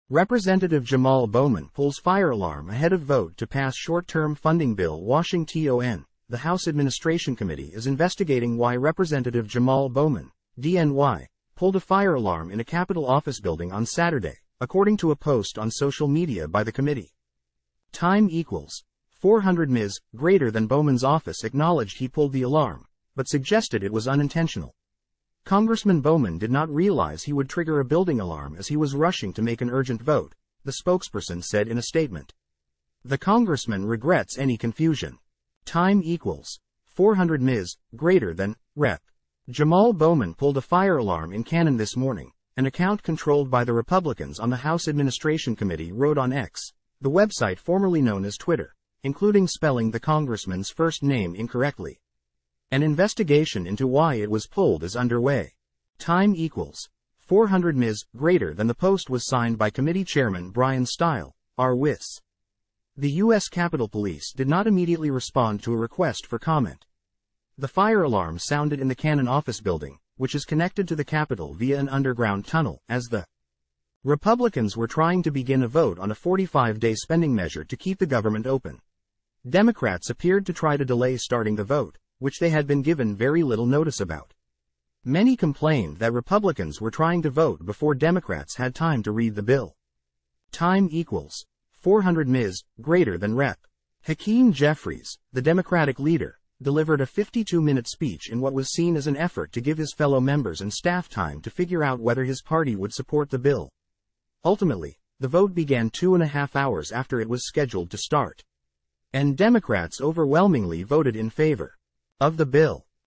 Rep. Jamaal Bowman pulls fire alarm ahead of vote to pass shortterm funding bill
Rep. Jamaal Bowman pulls fire alarm ahead of vote to pass short-term funding bill
WASHINGTON -- The House Administration Committee is investigating why Rep. Jamaal Bowman, D-N.Y., pulled a fire alarm in a Capitol office building on Saturday, according to a post on social media by the committee.

Bowman's office acknowledged he pulled the alarm, but suggested it was unintentional.

"Congressman Bowman did not realize he would trigger a building alarm as he was rushing to make an urgent vote," the spokesperson said in a statement. "The Congressman regrets any confusion."

"Rep. Jamal Bowman pulled a fire alarm in Cannon this morning," an account controlled by the Republicans on the House Administration Committee wrote on X, the website formerly known as Twitter, including spelling the congressman's first name incorrectly. "An investigation into why it was pulled is underway."

The post was signed by committee Chairman Bryan Steil, R-Wis.

The U.S. Capitol Police did not immediately respond to a request for comment.

The fire alarm sounded in the Cannon office building, which is connected to the Capitol via an underground tunnel, as the Republicans were trying to begin a vote on a 45-day spending measure to keep the government open.

Democrats appeared to try to delay starting the vote, which they had been given very little notice about. Many complained that Republicans were trying to vote before Democrats had time to read the bill.

Rep. Hakeem Jeffries, the Democratic leader, delivered a 52-minute speech in what was seen as an effort to give his fellow members and staff time to figure out whether his party would support the bill.

Ultimately, the vote began two and a half hours after it was scheduled to start. And Democrats overwhelmingly voted in favor of the bill.